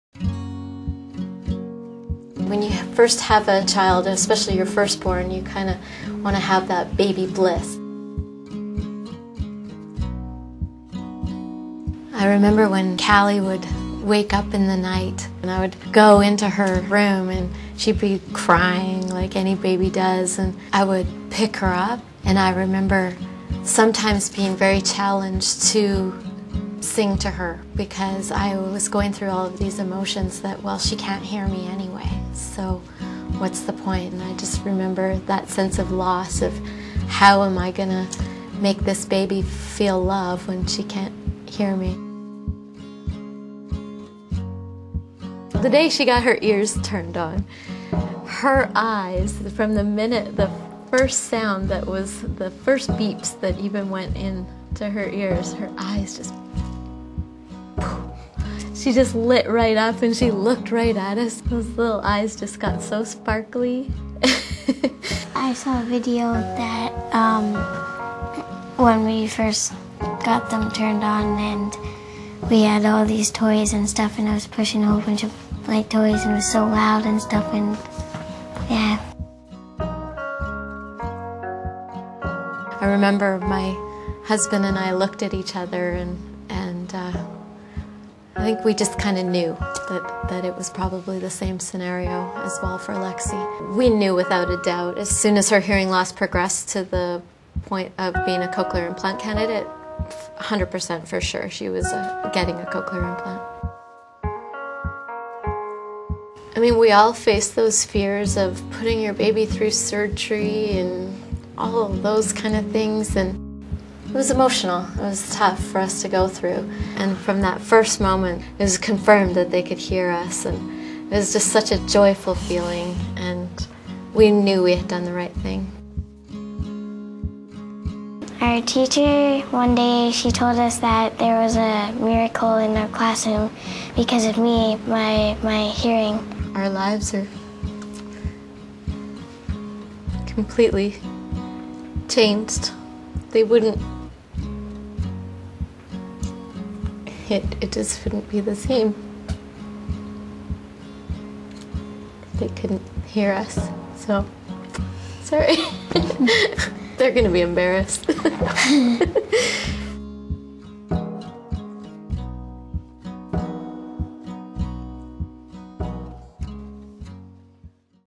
Young Sisters Born Deaf Describe Cochlear Implant Activation, Hearing for the First Time
to learn more about cochlear implant candidacy and connect with others with hearing loss experience.

On Mother's Day 2002, Dacy and Tim discovered that their first born, Callie, was born deaf. Dacy sang to Callie but found herself questioning how she could make the baby feel loved when Callie could not hear her. The first day that Callie had her cochlear implant activated, her eyes were "sparkly" with excitement and wonder. She was 12 months old. When Dacy and Tim had their second child, Lexie, they found out she also suffered from severe hearing loss. The Zacharias family verified their daughter's cochlear implant candidacy and activated her hearing implant without hesitation. In February 2005, Lexie was able to hear her parents' voices for the first time. Learn how the Zacharias family discovered sound with the help of Cochlear.

to discover additional Cochlear recipient stories to learn more.